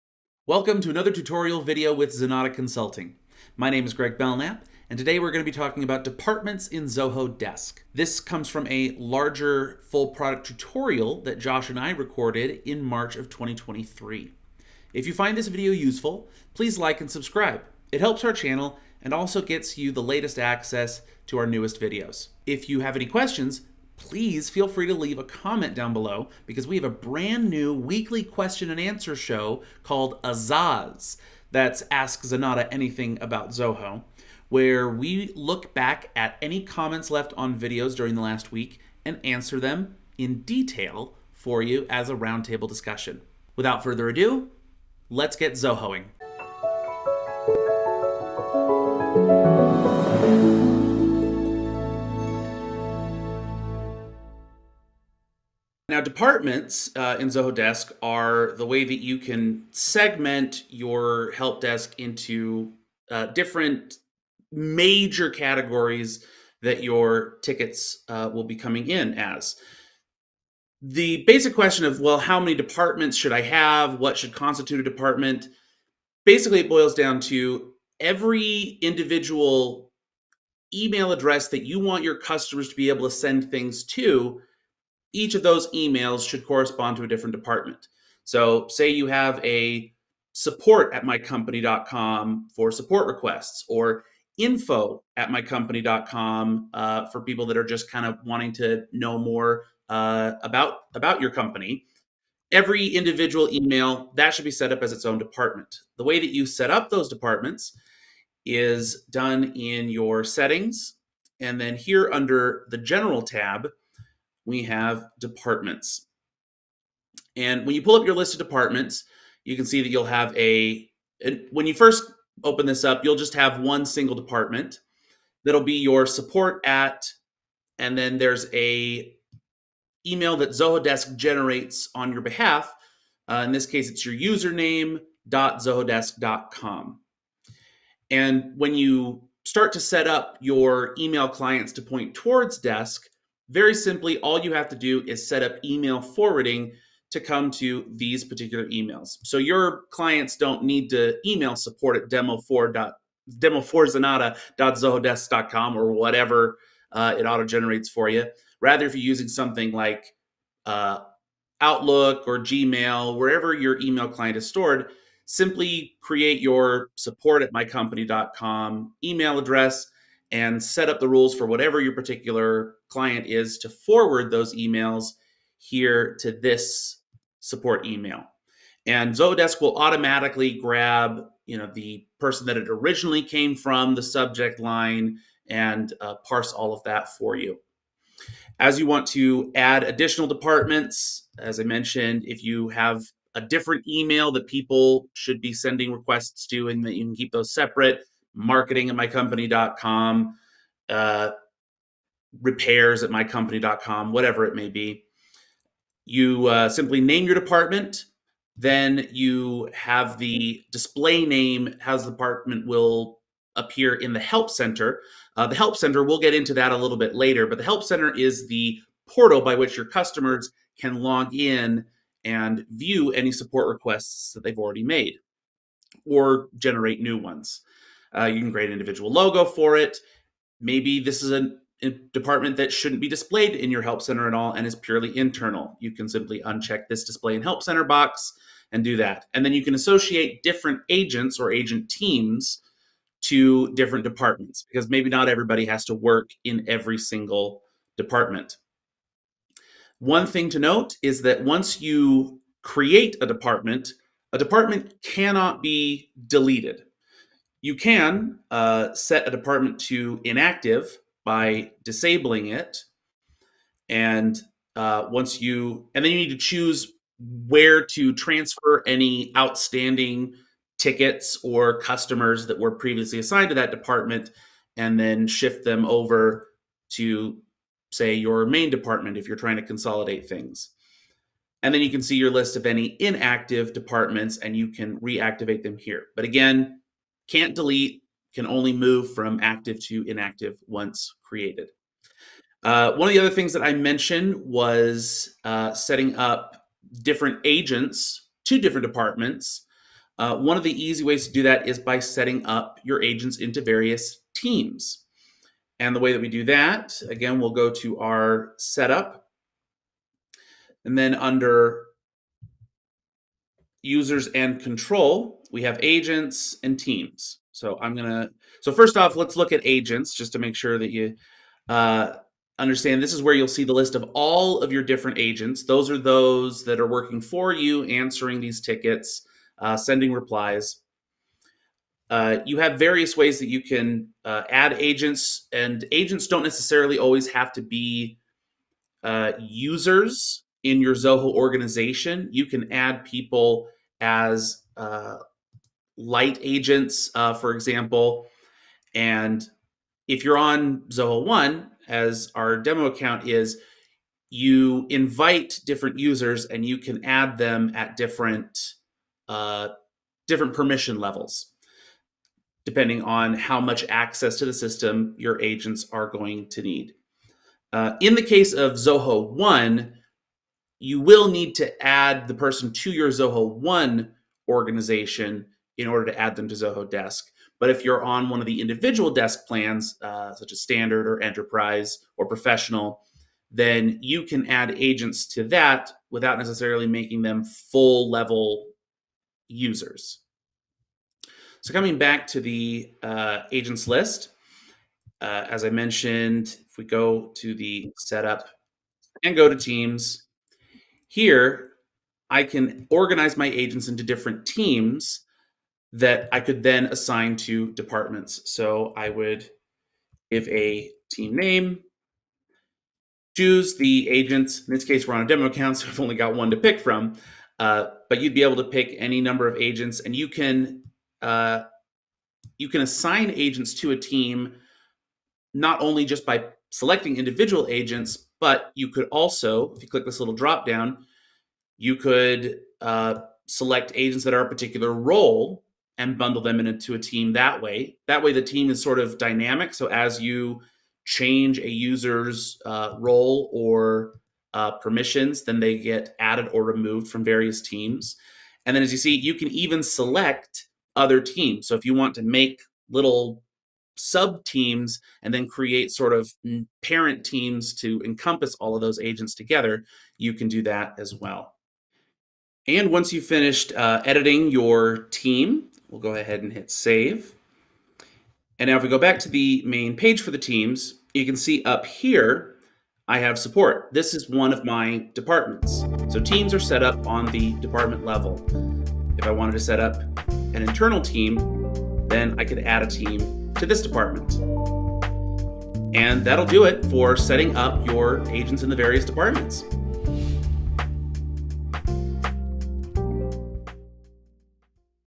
Setting Up Departments in Zoho Desk With Email Forwarding & Agent Teams
This tutorial on setting up departments in Zoho Desk with email forwarding & agent teams was taken from our March 2023 product tutorial series.

Zoho One | All 46+ Apps Explained
January 21, 2025, 10:00 AM Pacific Standard Time